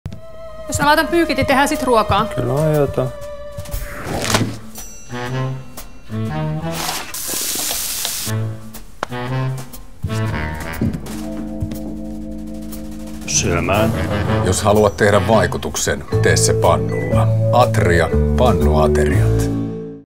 Atria Pannuateriat – tee vaikutus pannulla
Uudet Atria Pannuateriat tuovat Intian makuja lautasellesi vain 10 minuutissa. Pannuaterioista löytyy kaksi herkullista makua: Butter Chicken ja Tandoori Chicken. Molemmat sisältävät suomalaista kanaa ja kasviksia sekä intialaisen maustekastikkeen. Lisää vain ruoka- tai kaurakerma ja nauti haluamasi lisukkeen kanssa.